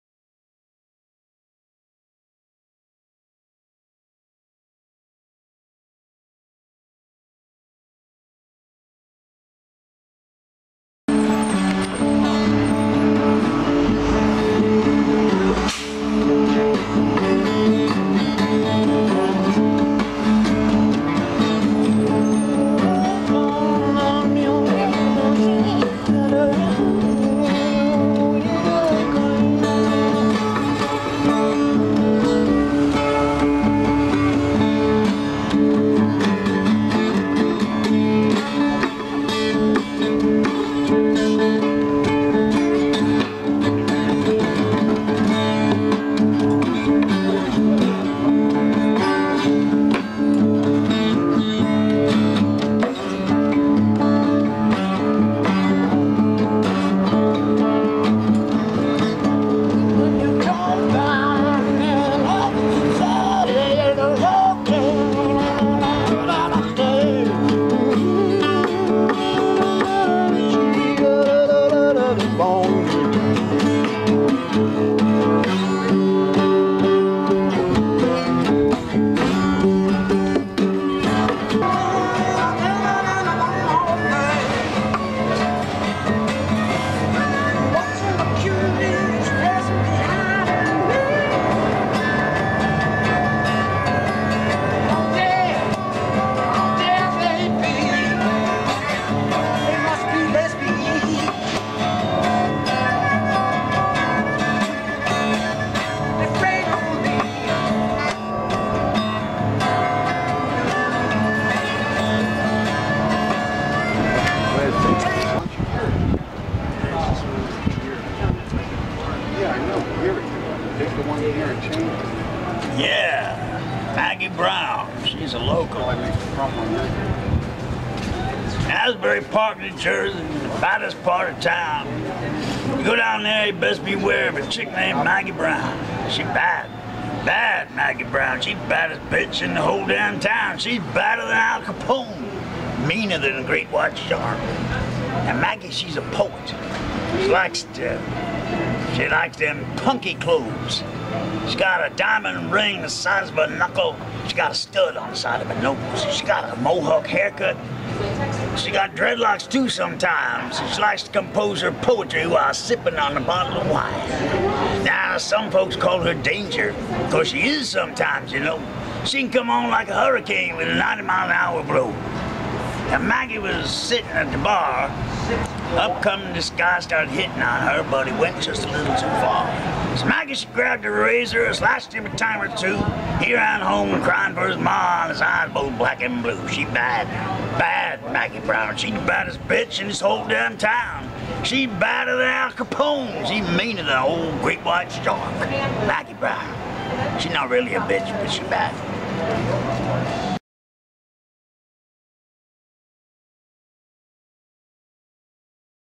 AMERICAS CUP
AMERICAS CUP Coffee shop...culture rears it's ugly head in Asbury Park with spontaneous music and poetry...natural to all good coffee shops,  not to mention great coffee